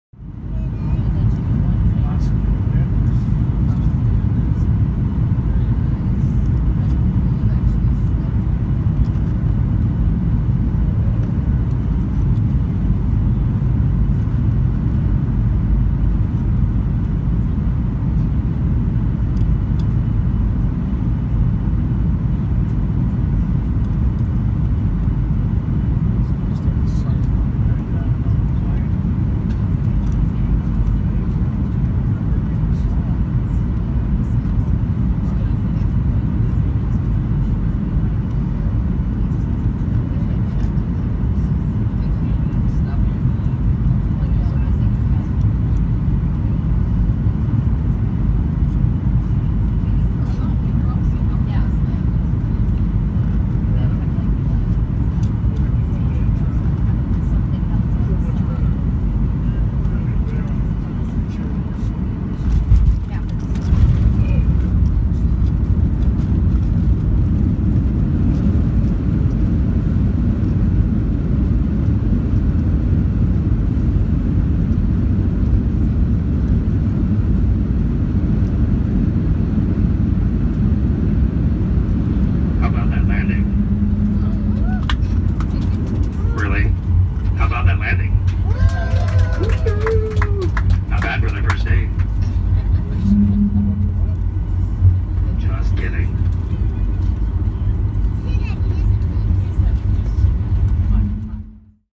Southwest 737-800 Smooth Landing! Funny Announcement | Denver, CO
Found a video of a landing in Denver, CO from March 2018.